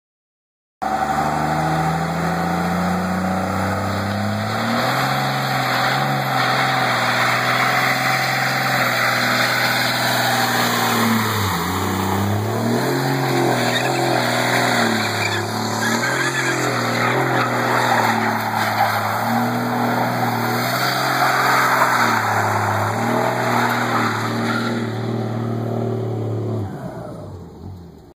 Dodge 2500 Cummins Burnout
New daily driver/play truck. Giving it a thorough test run.